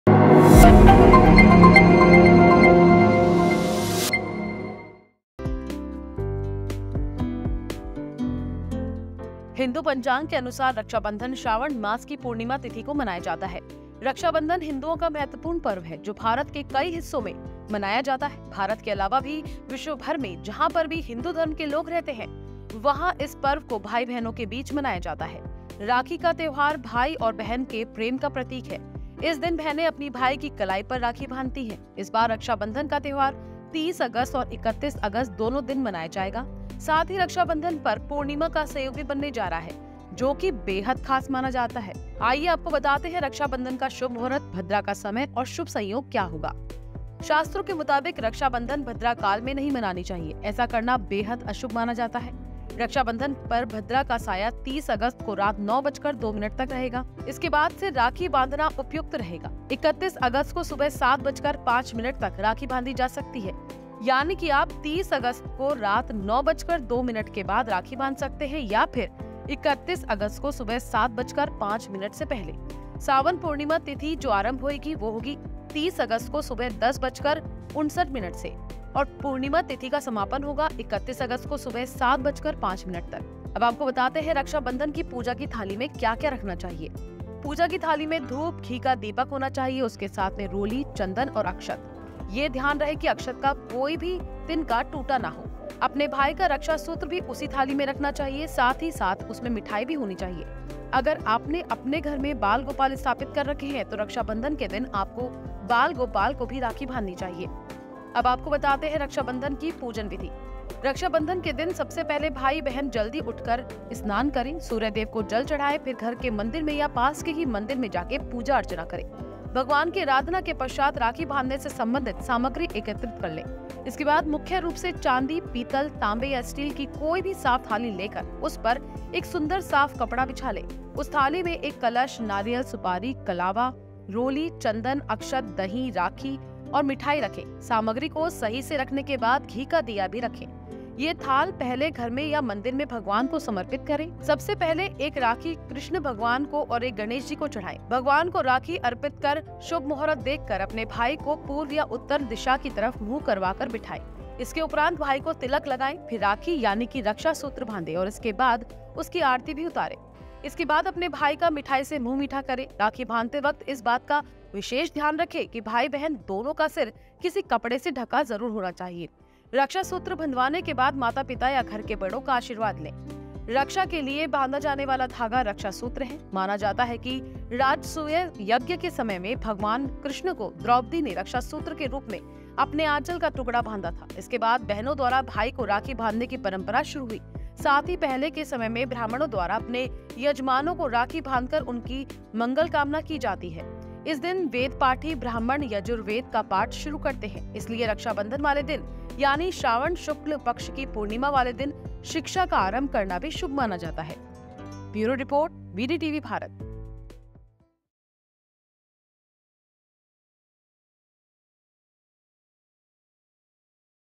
30 या 31 August कब होगा Raksha Bandhan, भद्रा के समय भूल कर भी ना बांधे राखी | Rakhi Muhurat 2023
हिंदू पंचांग के अनुसार, रक्षाबंधन श्रावण मास की पूर्णिमा तिथि को मनाया जाता है. रक्षाबंधन हिंदुओं का महत्वपूर्ण पर्व है, जो भारत के कई हिस्सों में मनाया जाता है. भारत के अलावा भी विश्वभर में जहां पर भी हिंदू धर्म के लोग रहते हैं, वहां इस पर्व को भाई बहनों के बीच मनाया जाता है. राखी का त्योहार भाई और बहन के प्रेम का प्रतीक है. इस दिन बहनें अपनी भाई की कलाई पर राखी बांधती हैं. इस बार रक्षाबंधन का त्योहार 30 अगस्त और 31 अगस्त दोनों दिन मनाया जाएगा. साथ ही रक्षाबंधन पर पूर्णिमा का संयोग भी बनने जा रहा है जो कि बेहद खास माना जाता है. आइए जानते हैं रक्षाबंधन का शुभ मुहूर्त, भद्रा का समय और शुभ संयोग.

शास्त्रों के मुताबिक, रक्षाबंधन भद्रा काल में नहीं मनानी चाहिए, ऐसा करना बेहद अशुभ माना जाता है.
रक्षाबंधन पर भद्रा का साया 30 अगस्त को रात 9 बजकर 2 मिनट तक रहेगा, इसके बाद से राखी बांधना उपयुक्त रहेगा. 31 अगस्त को सुबह 7 बजकर 5 मिनट तक राखी बांधी जा सकती है.
यानी कि आप 30 अगस्त को रात 9 बजकर 2 मिनट के बाद राखी बांध सकते हैं या फिर 31 अगस्त को सुबह 7 बजकर 5 मिनट से पहले.
सावन पूर्णिमा तिथि आरंभ- 30 अगस्त को सुबह 10 बजकर 59 मिनट से
पूर्णिमा तिथि समापन- 31 अगस्त को सुबह 7 बजकर 5 मिनट तक

रक्षाबंधन की पूजा की थाली
 पूजा की थाली में धूप, घी का दीपक होना चाहिए. उसमें रोली और चंदन रखें. उसमें अक्षत रखना चाहिए यानी चावल जो टूटा हुआ न हो. अपने भाई का रक्षा सूत्र भी उसी थाली में रखना है, साथ ही साथ उसमें मिठाई भी रखनी है. अगर आपने अपने घर में बाल गोपाल स्थापित कर रखें हैं तो रक्षाबंधन के दिन आपको बाल गोपाल को भी राखी बांधनी चाहिए.

 रक्षाबंधन पूजन विधि

रक्षा बंधन के दिन सबसे पहले भाई बहन जल्दी सुबह उठकर स्नान आदि कर, सूर्य देव को जल चढ़ाएं. फिर घर के मंदिर में या पास ही के मंदिर जाकर पूजा अर्चना करें. भगवान की आराधना के पश्चात राखी बांधने से संबंधित सामग्री एकत्रित कर लें. इसके बाद मुख्य रूप से चांदी, पीतल, तांबे या स्टील की कोई भी साफ थाली लेकर उस पर एक सुंदर-साफ कपड़ा बिछा लें. उस थाली में एक कलश, नारियल, सुपारी, कलावा, रोली, चंदन, अक्षत, दही, राखी और मिठाई रख लें. सामग्री को सही से रखने के बाद घी का दीया भी रखें.
यह थाल पहले घर में या मंदिर में भगवान को समर्पित करें. सबसे पहले एक राखी कृष्ण भगवान और एक गणेश जी को चढ़ाएं. भगवान को राखी अर्पित कर ऊपर बताए गए शुभ मुहूर्त देख अपने भाई को पूर्व या उत्तर दिशा की तरफ मुंह करवाकर बिठाएं. इसके उपरान्त भाई को तिलक लगाएं, फिर राखी यानी रक्षा सूत्र बांधें और इसके बाद उसकी आरती करें. इसके बाद अपने भाई का मिठाई से मुंह मीठा करें. राखी बांधते वक्त इस बात का विशेष ध्यान रखें कि भाई-बहन दोनों का सिर किसी कपड़े से ढका जरूर होना चाहिए. रक्षा सूत्र बंधवाने के बाद माता-पिता या घर के बड़ों का आशीर्वाद लें.
रक्षा के लिए बांधा जाने वाला धागा रक्षासूत्र है. माना जाता है कि राजसूय यज्ञ के समय में भगवान कृष्ण को द्रोपदी ने रक्षासूत्र के रूप में अपने आंचल का टुकड़ा बांधा था. इसके बाद बहनों द्वारा भाई को राखी बांधने की परंपरा शुरू हुई. साथ ही पहले के समय में ब्राह्मणों द्वारा अपने यजमानों को राखी बांधकर उनकी मंगलकामना की जाती है. इस दिन वेदपाठी ब्राह्मण यजुर्वेद का पाठ शुरू करते हैं. इसलिए रक्षाबंधन वाले दिन यानी श्रावण शुक्ल पक्ष की पूर्णिमा वाले दिन शिक्षा का आरंभ करना भी शुभ माना जाता है.